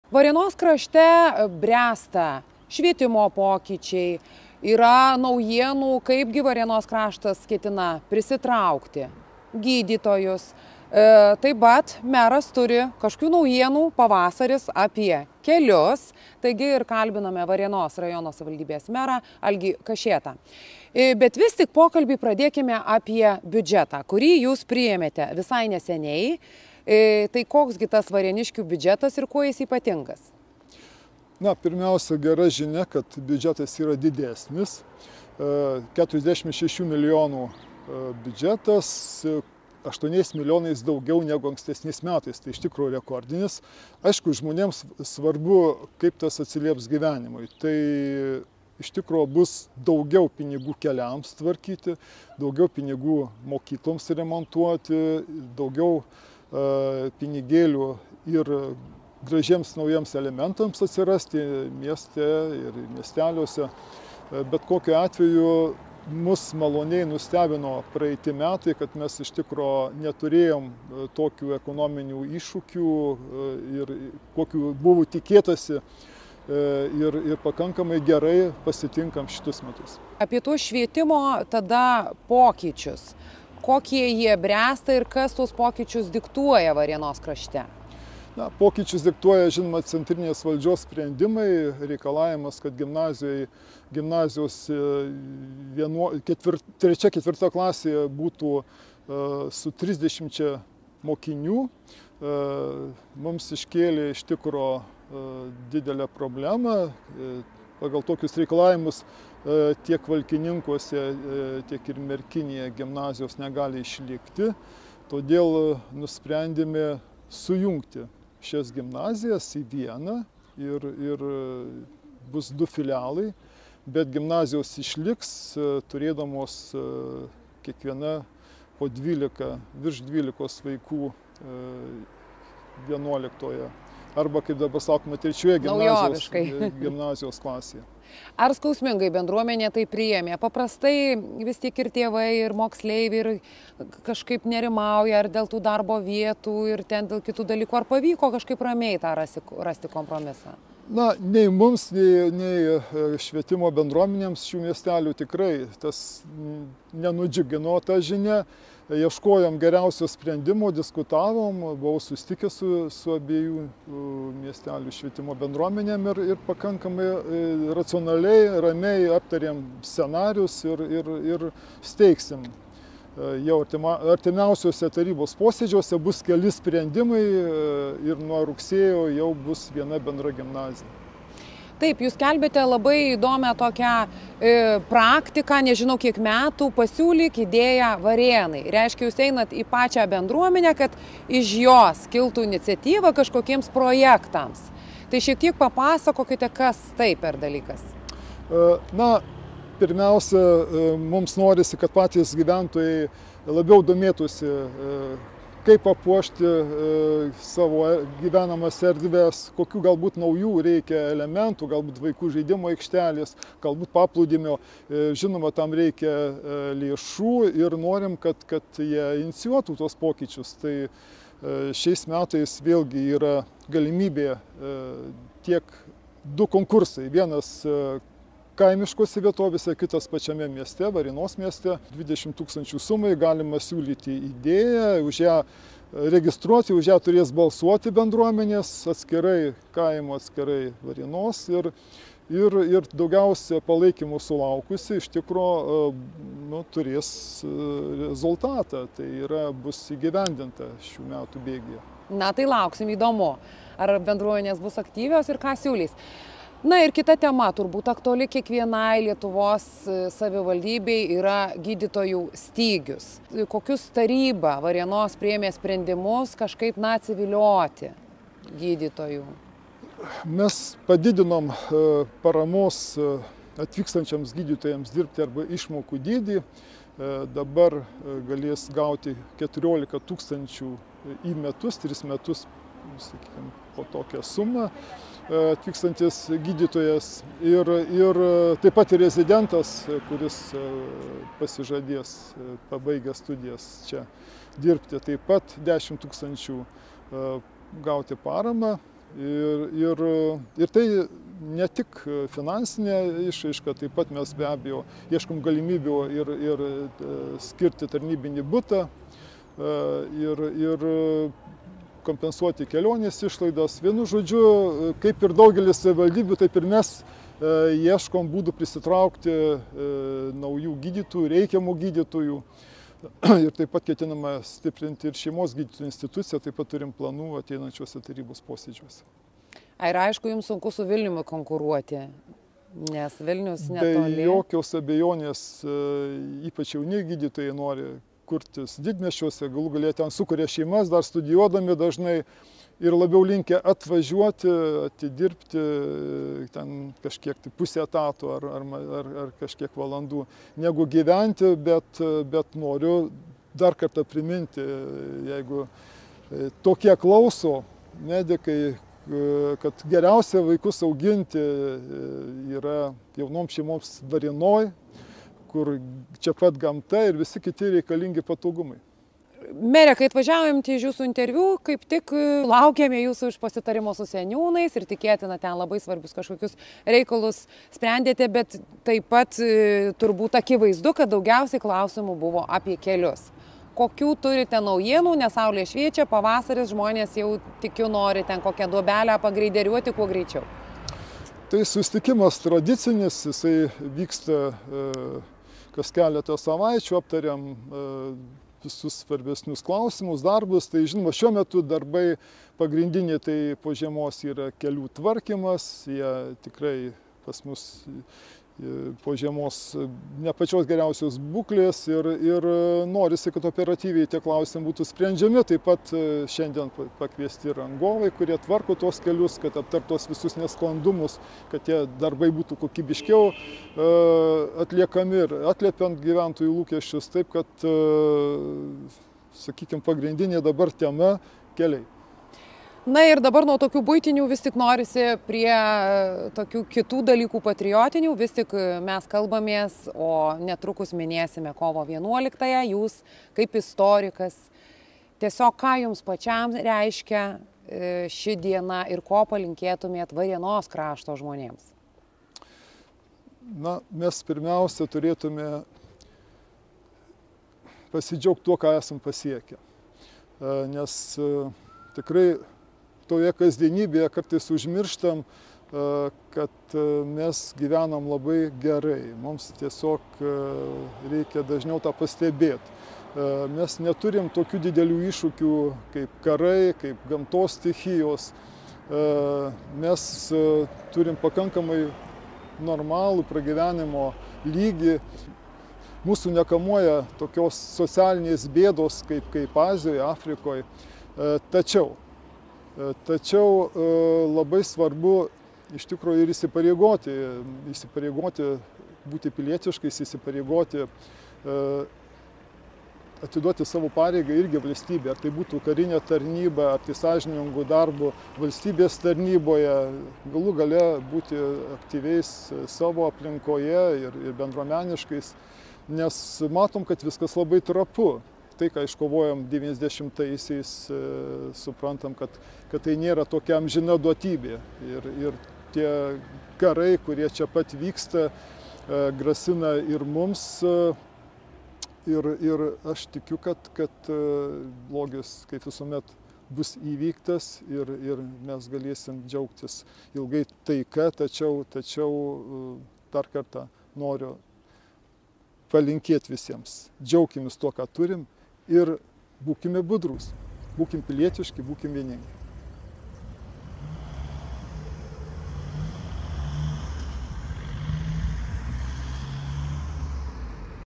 Varėnos meras. Apie iššūkius švietime ir geras naujienas dėl kelių
Šių metų Varėnos rajono savivaldybės biudžetas - 46 mln. Eur. apimties. „Turime pasidžiaugti, nes šiais metais biudžetas 8 mln. Eur didesnis. O tai reiškia, kad daugiau bus skirta pinigų keliams, mokykloms remontuoti, naujiems elementams ūkiniuose reikaluose, kultūros gyvenime. Maloniai nustebino praėjusieji metai, nes neturėjome ekonominių iššūkių, tad finansiškai jie pabaigti gerai“, - kalbėjo Varėnos rajono savivaldybės meras Algis Kašėta.

Tiesa, Varėnoje bręsta pokyčiai švietimo srityje, kuriuos diktuoja centrinės valdžios sprendimai. Savivaldybės vadovai kartu su taryba ir administracija, ieško būdų ir galimybių, kaip pritraukti gydytojų, kurių trūksta.

Apie visa tai ir artėjančią kovo 11 - ąją, ko Varėnos rajono meras linkti savo ir visos šalies gyventojams - pokalbis su A. Kašėta - videoreportaže.